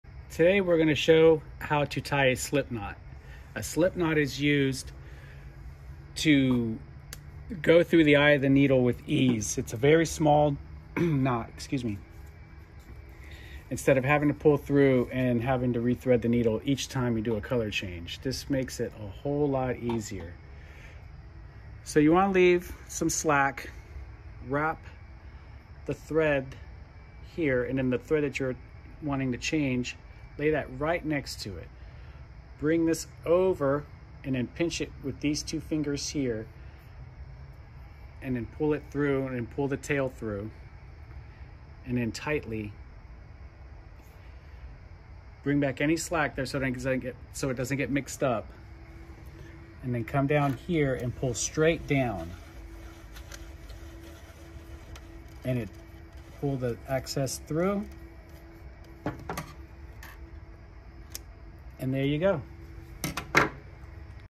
How to tie a Slipknot! | Sunbeam Embroidery Tutorial
In this tutorial we show the steps in tying a Slipknot. This trick will explain how to make the Slipknot go through the needle with ease.